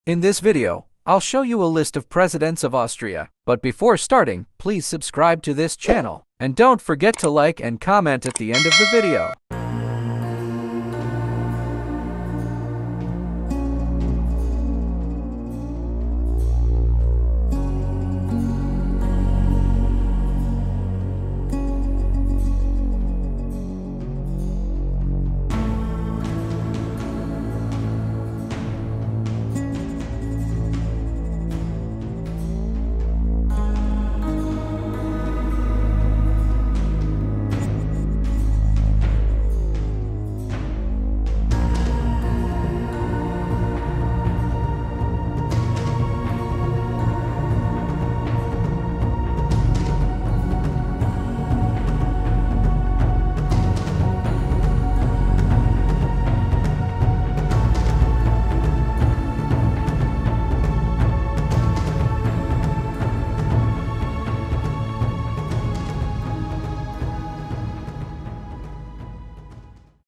List of President of Austria
In this video, we'll learn about the first president of the Ghana until now, Karl Renner.